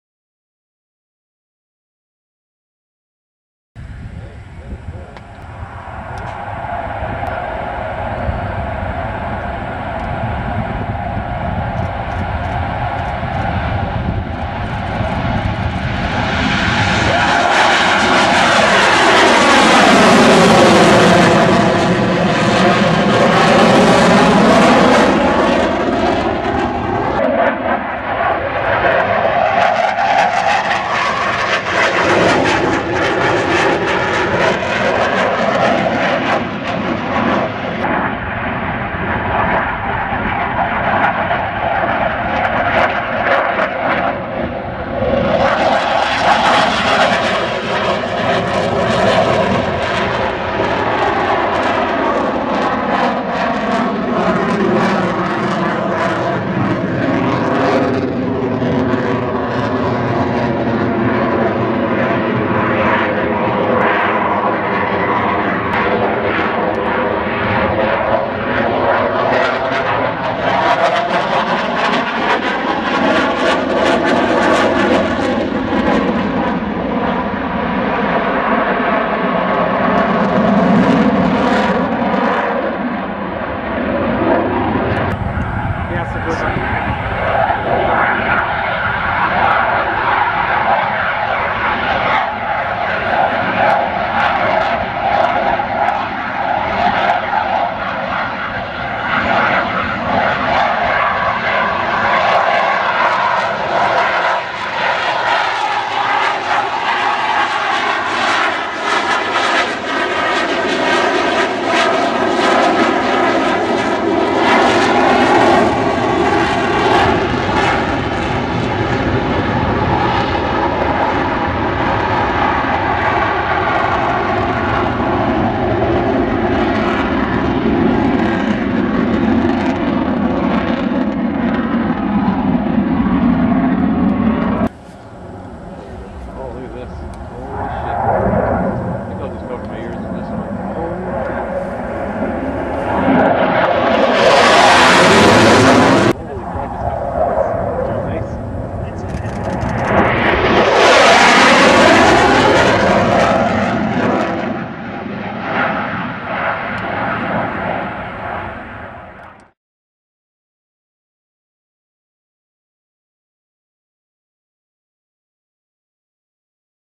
Wings and Wheels 2009 CF-18 Demo
This is most of the CF-18 demo recorded at Wings and Wheels 2009 on Friday Arrival day. Such a Beautiful Paint Scheme and Donor is a helluva hornet driver!